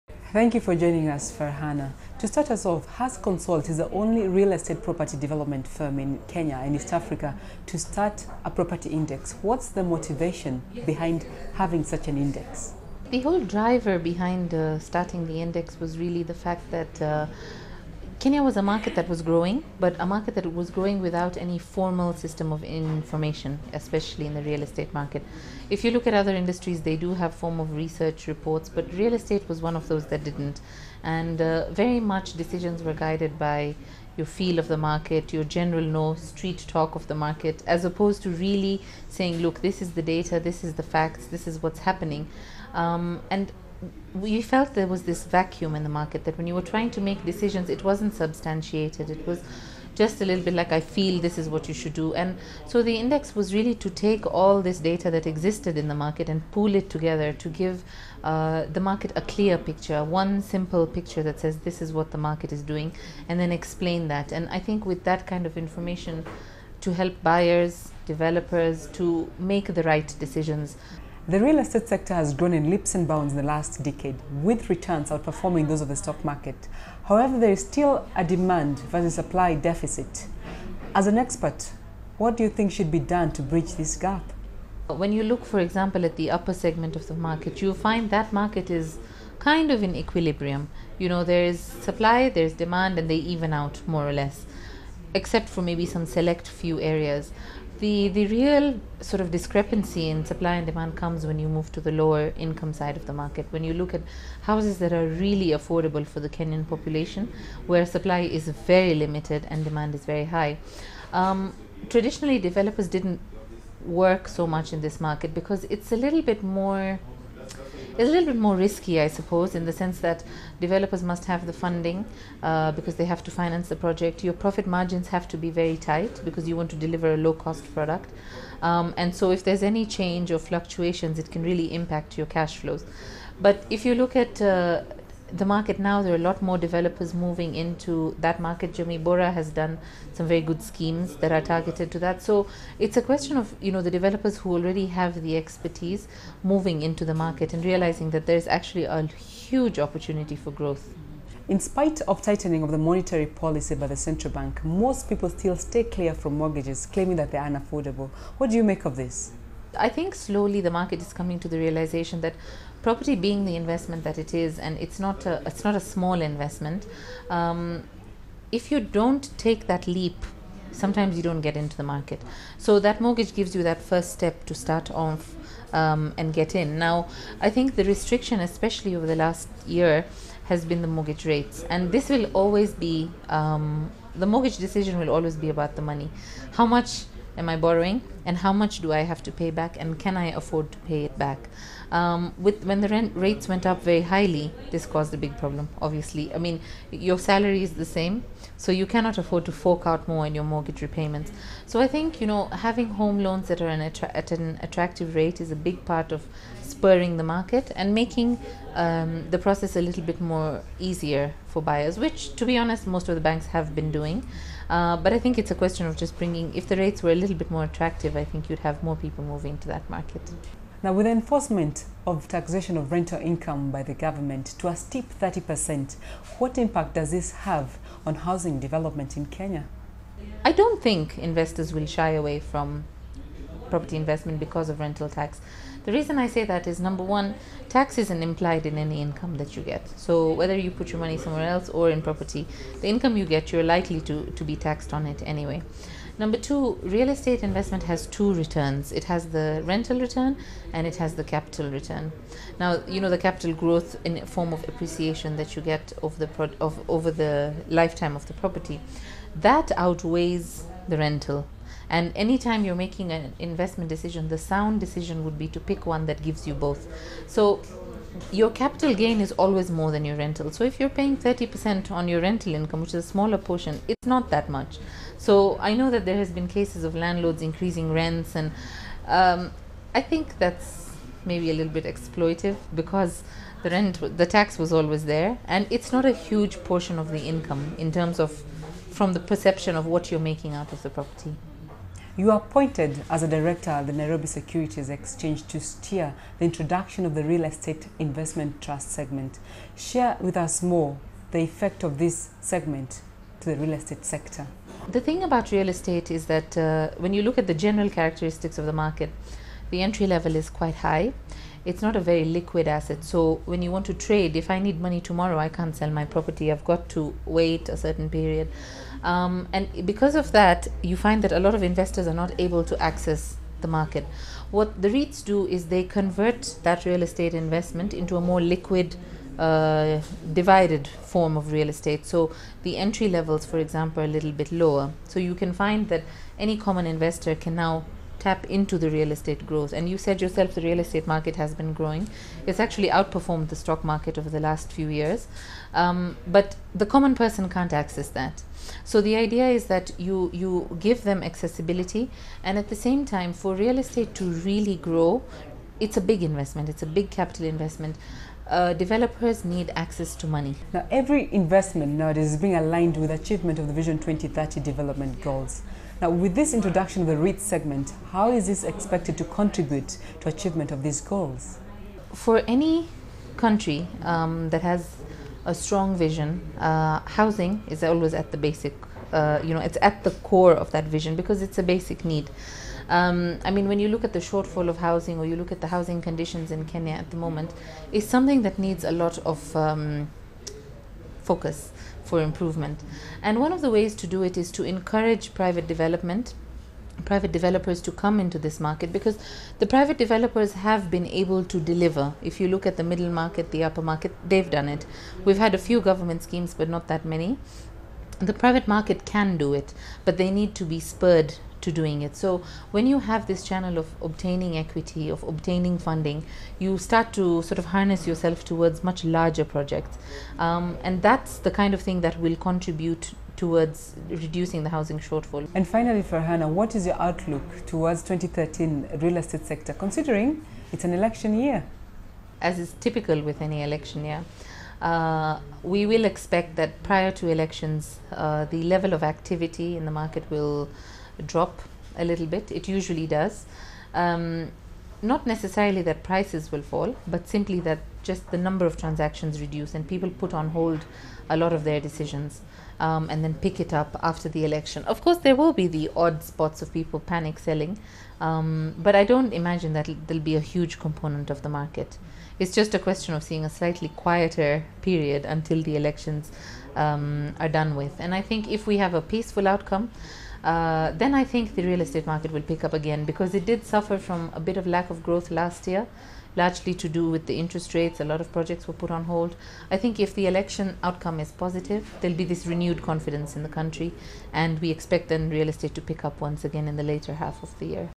Pacesetter: Hass Consult, CEO of Farhana Hassanali
Kenya's real estate industry has been growing ten-fold in the last decade with returns outperforming those of the Nairobi securities exchange. Loise Wachira caught up with Farhana Hassanali from Hass Consult and appointed director of the Nairobi securities exchange Real Estate Investment trust Segment for insights on this sector.